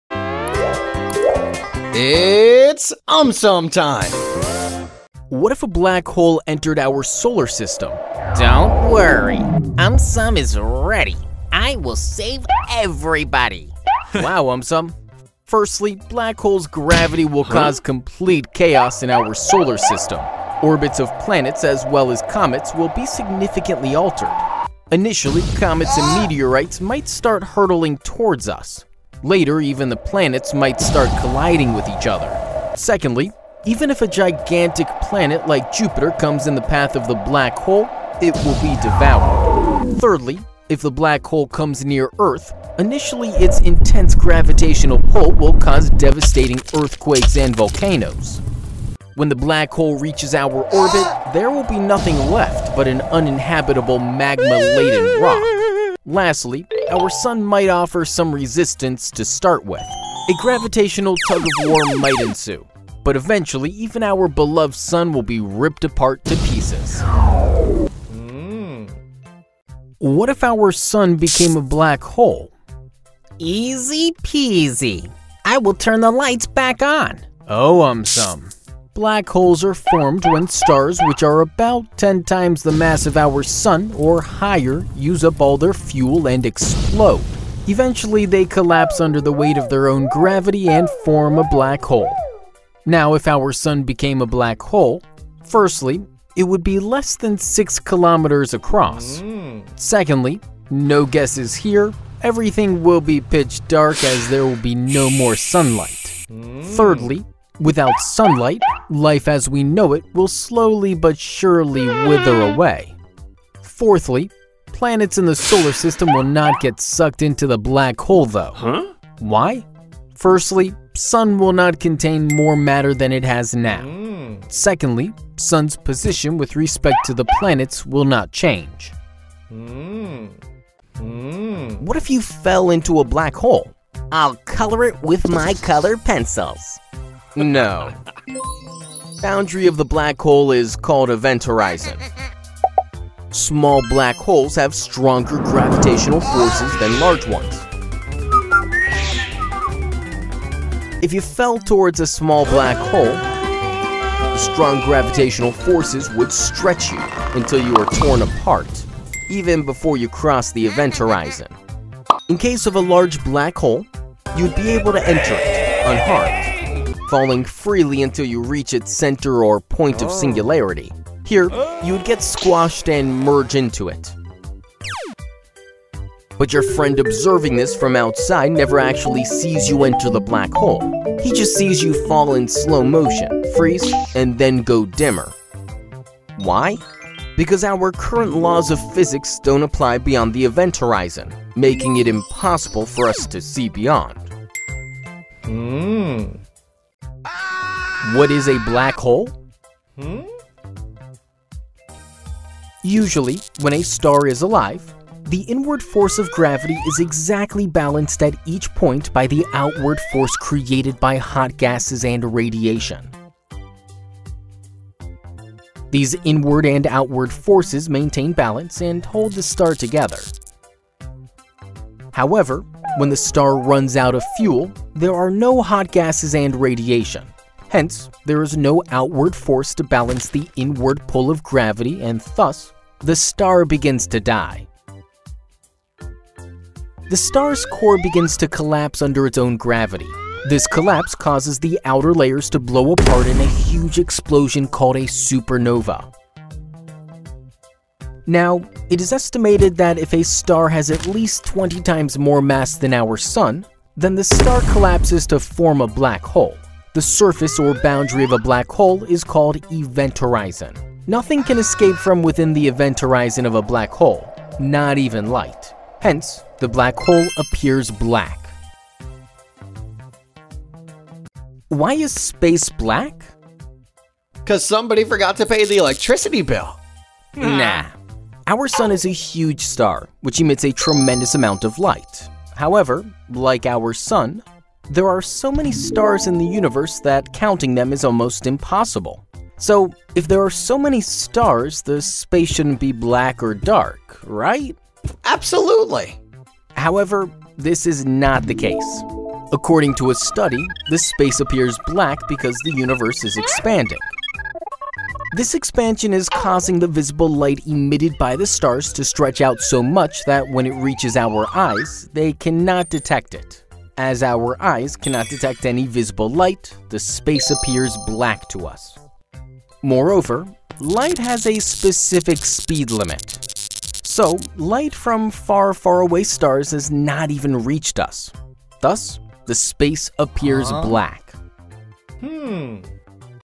What If a Black Hole Threatened Our Solar System? + more videos
Firstly, Black hole's gravity will cause complete chaos in our Solar System. Orbits of Planets as well as Comets will be significantly altered. Initially comets and meteorites might start hurtling towards us. Later even the planets might start colliding with each other.
Secondly, even if a gigantic planet like Jupiter comes in the path of the Black hole it will be devoured.
Thirdly, if the Black hole comes near Earth, initially its intense gravitational pull will cause devastating earthquakes and volcanoes. When the Black hole reaches our orbit there will be nothing left but an uninhabitable magma-laden rock.
Lastly, our Sun might offer some resistance to start with. A gravitational tug of war might ensue. But eventually even our beloved Sun will be ripped apart to pieces.